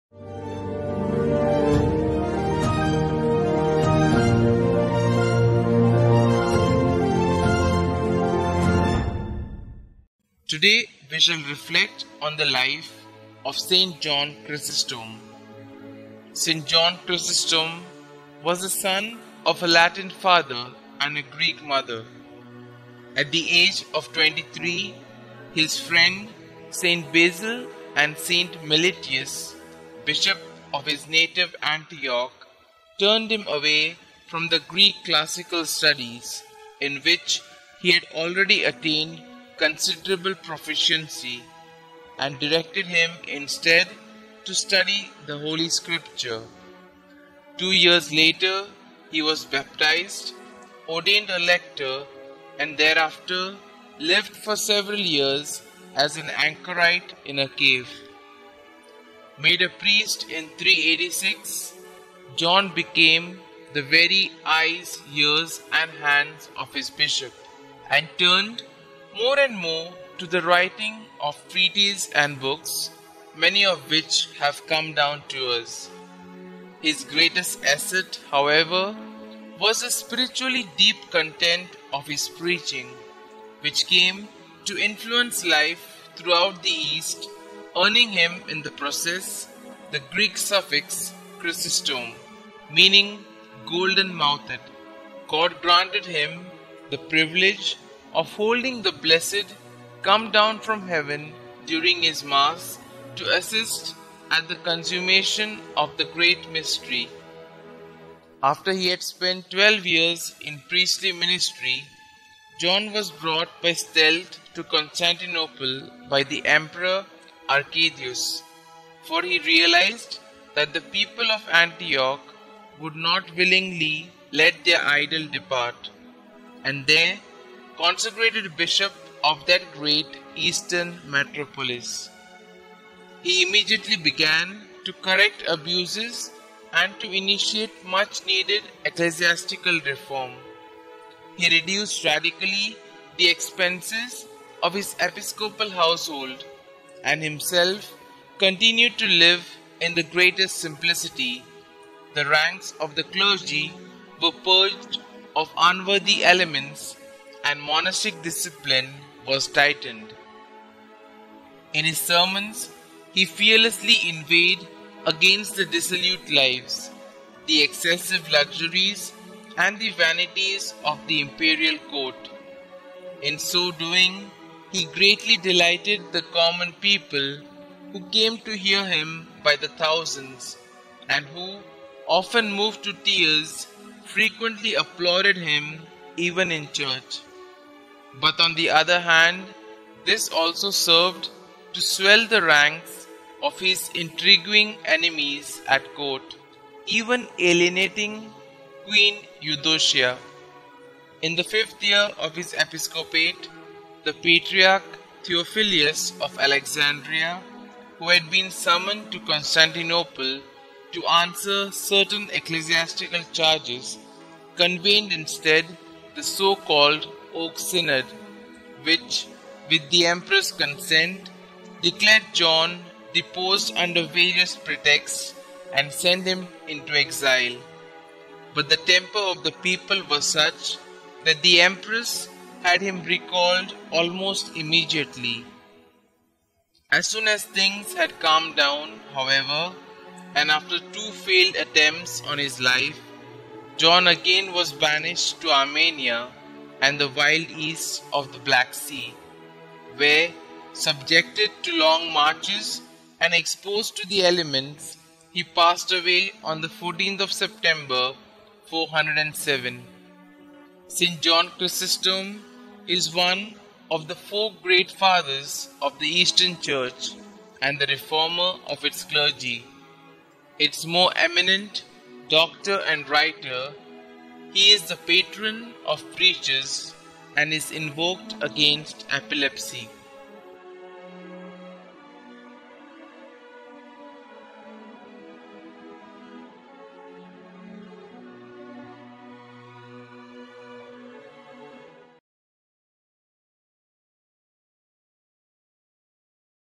St John Chrysostom - Saint of the Day with Fr Lindsay - 13 Sept 2022
Here is a brief reflection on the life of St John Chrysostom presented by Fr Lindsay Lobo.